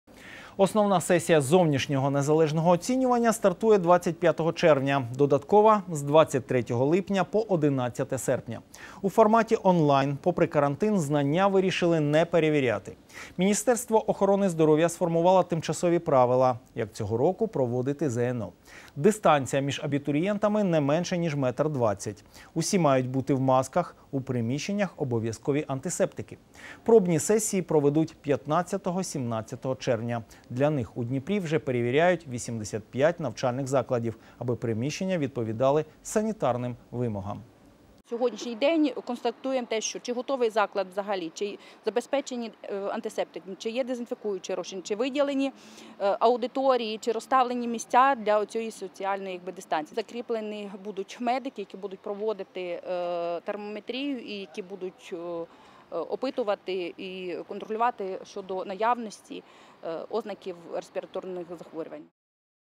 Для проведення ЗНО Міністерство охорони здоров’я сформувало тимчасові правила
Основна сесія зовнішнього незалежного оцінювання стартує 25 червня. Додаткова - з 23 липня – по 11 серпня. У форматі он-лайн, попри карантин, знання вирішили не перевіряти. Міністерство охорони здоров’я сформувало  тимчасові правила, як цього року проводити ЗНО. Дистанція між абітурієнтами, не менше ніж метр двадцять, усі мають бути в масках, у приміщеннях обов'язкові антисептики. Пробні сесії проведуть 15-17 червня. Для них у Дніпрі вже перевіряють 85 навчальних закладів, аби приміщення відповідали санітарним вимогам.